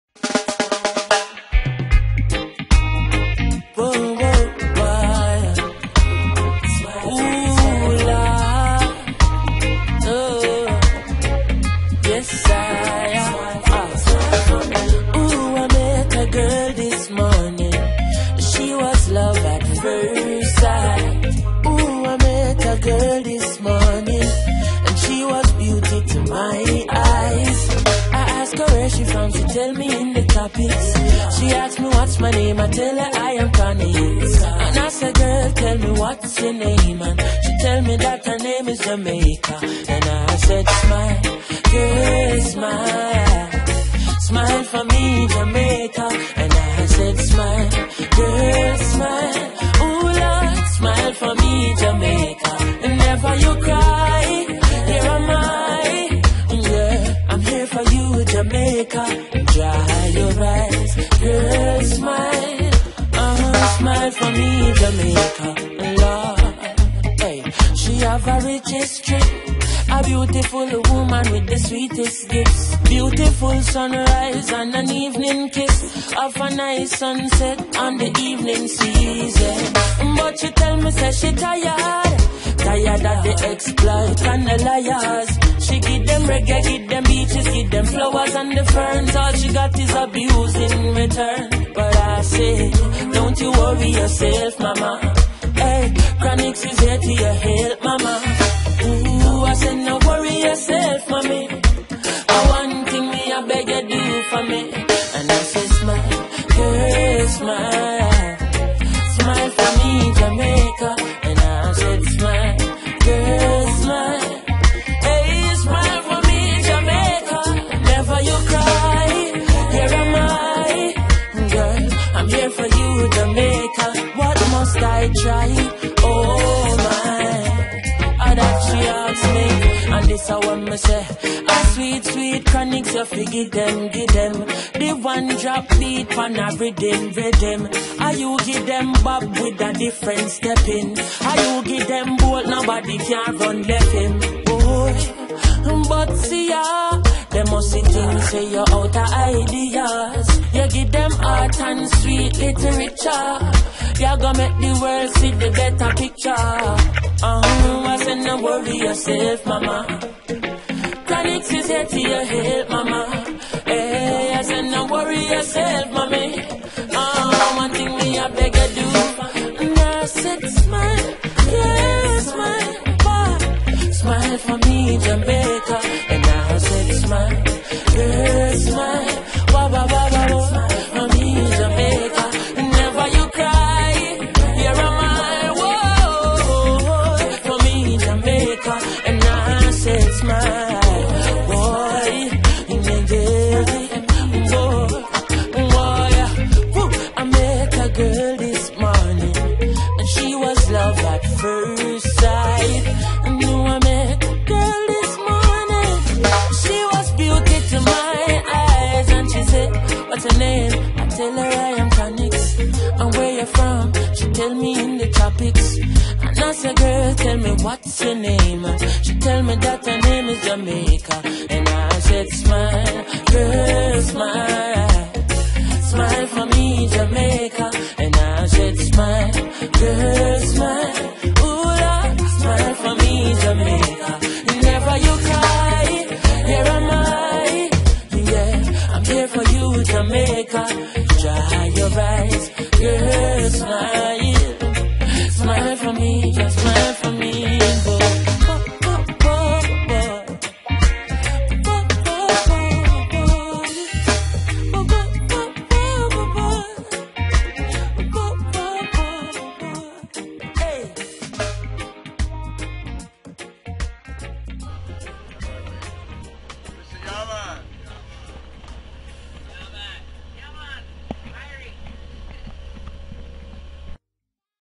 7 Mile Negril Jamaica Snorkel Trip
Our awesome trip to West End Negril with The House's! Watch to the end to see the cool sea turtle!